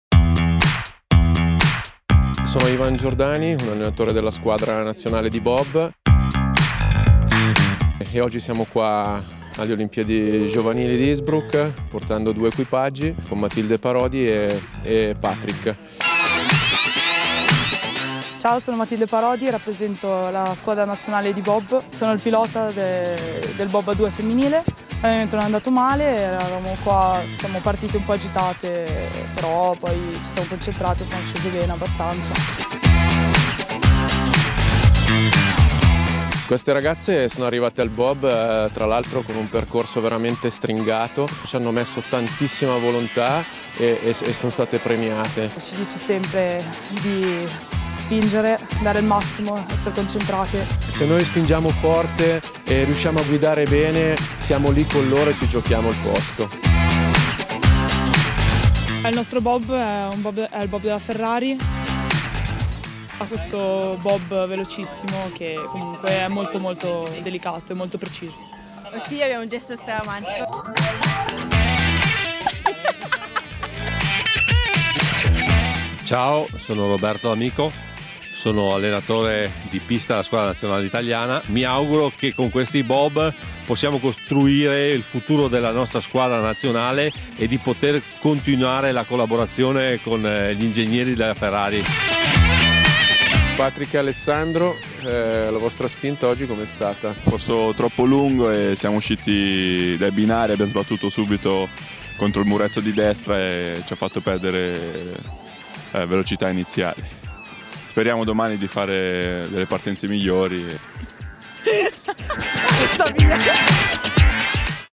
Il mio amico Bob - ITALIA TEAM INNSBRUCK
Fra allenamenti e strigliate dell'allenatore, vediamo i ragazzi del team italiano di Bob che si preparano ad affrontare le gare ad Innsbruck.
Ivan Giordani racconta la passione e la fatica per arrivare qui, ma anche la determinazione che hanno i ragazzi. Mentre Roberto D'amico racconta del particolare Bob prodotto dalla Ferrari che il team italiano ha in dotazione.